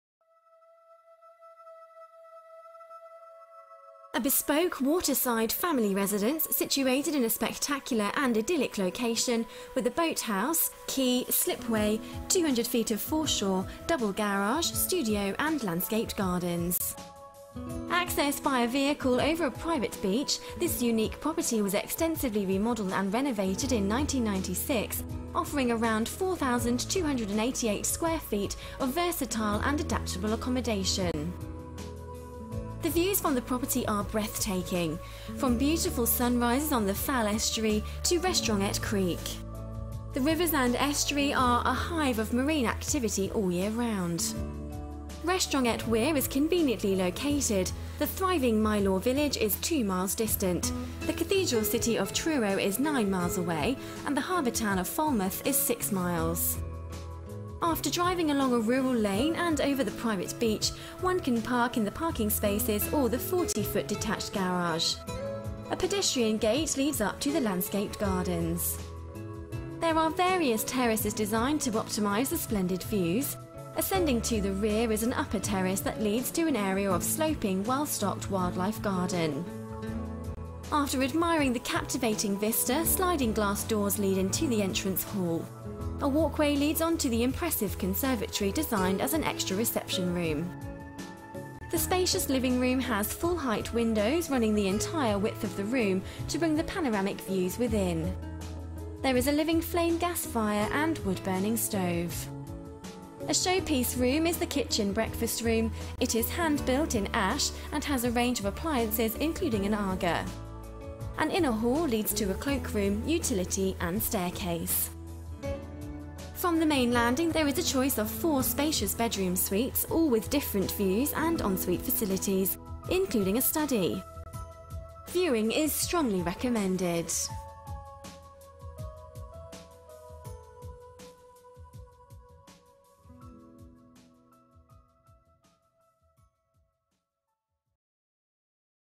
SOLD BY H TIDDY Sun Rising, Restronguet Weir, Truro, Cornwall.
SIMILAR PROPERTIES REQUIRED!

An unrivalled opportunity to acquire a spacious and versatile waterside family residence, situated in a spectacular and yet idyllic location, with boat house, quay, slipway, foreshore, gardens and double garage with studio, all literally accessed by vehicle over a private beach!

Location summary – (distances and times are approximate)

Truro – 9 miles.  Falmouth – 6 miles.  Mylor Bridge – 2 miles.  Newquay Airport – 28 miles.  Exeter – 96 miles.  Mylor Yacht Harbour – 3.5 miles by car (0.75 miles by water!)

• Viewing unhesitatingly recommended!
• Around 4288 sq ft of versatile accommodation.
• Recently built Boathouse with double glazed windows!
• Slipway and Quay.
• Captivating water and countryside views.
• Circa 200ft foreshore to low water springs.
• Mooring rights and deep-water access!
• Large detached double garage with first floor studio.
• Sloping, well-stocked gardens with sun terraces.
• 4 bedrooms, all with en suite bathrooms.
• Extensively renovated and remodelled in 1996.
• 39'5 x 25'11 living room.
• 25' kitchen / breakfast room.
• 39'5 master bedroom suite.
• Two further guest bedroom suites.
• Roof and sun terraces offering the breathtaking views.
• Deceptively spacious, light and airy accommodation.
• Been in the ownership of the family for over 45 years.
• Convenient yet unrivalled situation.
• Vehicular access over a private beach.
• The sailing / water sports enthusiasts dream.

Location – Restronguet Weir

Occupying an idyllic, peaceful situation on the eastern banks of the Fal Estuary, Sunrising is found off a no-through road in an unassuming and partially elevated position.  This exclusive waterside hamlet offers amazing water views that have to be seen first hand to be fully appreciated.  The property is located only about 2 miles from Mylor Bridge, which is a thriving village community with amenities including a range of local shops to cater for everyday needs.  These include a post office stores, general stores with off license, green grocer, butcher, newsagents, doctors surgery, primary school, local bus service and the Lemon Arms Public House.  Mylor Yacht Harbour is about 3.5 miles away by car or 0.75 miles by water, which has a popular sailing club, a variety of café / restaurants and deep-water moorings as well as pontoon berths.

The Cathedral City of Truro and Falmouth Harbour Town are readily accessible (9 miles and 6 miles respectively) both of which have a wide range of amenities including railway stations.  Truro has National mainline railway links.  London Paddington is around a 4.5 hour commute.  Newquay Airport is about 28 miles away and has regular daily flights to London Gatwick being around 70 minutes away.  The A30 dual carriageway joins the M5 at Exeter around 96 miles distant.

The area has a re-known micro climate and is best known for its sailing and all combinations of watersports from deep water fishing to kayaking.  For the keen walker and bird watcher, there are a variety of footpaths and walks nearby.  The Eden Project and a reputable number gardens such as Trelissick, Trebah, Glendurgan, Lanhydrock and Heligan, are all within easy distance.  For the keen golfer local courses include Falmouth, Killiow and Truro.  The area has excellent educational facilities, from sought after primary and secondary schools, with Truro offering a variety of private schools for all ages, whilst Truro College and Falmouth University at Tremough offer excellent facilities and courses for those in higher education.

Description

Sunrising was originally built in the 1920's where its thatched roof remained just a few years before our client's family purchased the property in the early 1960's.  The family fell in love with the location after renting the property for summer holidays for a few years prior to purchasing.

The property was extensively remodelled and extended with works commencing in 1996.  The detached boathouse and slipway was rebuilt a couple of years ago.

Our clients insisted that the build reflect the surroundings as well as capture the outstanding views via picture double glazed windows and sun terraces in all principal rooms, yet offer a combination of open plan formal and informal living.  Only by viewing internally can one fully appreciate the depth of family sized accommodation that caters, either as a permanent or second home residence, for the large family that is also versatile and easy enough to manage for the retiring couple.

The views from the property are breathtaking and ever changing.  From beautiful sun rises on the Fal Estuary to Restronguet Creek and Penpol, the estuary is a hive of marine activity all year round.